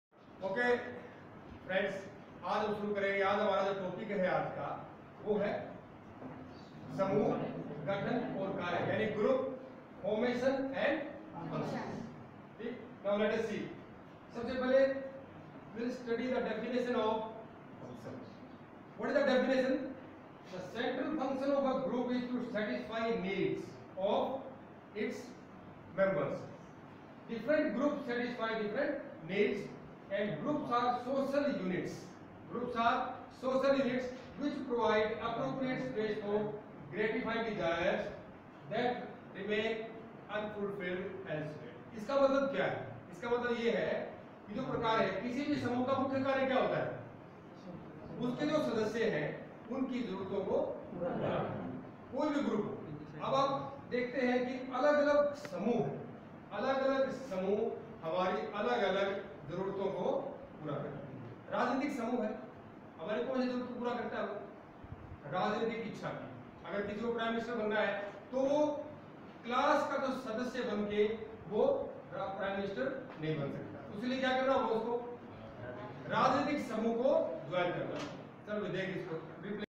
gcadampur Rajesh Verma Psychology- Group Formation and Functions
F.G.M. GOVT.COLLEGE, ADAMPUR (HISAR)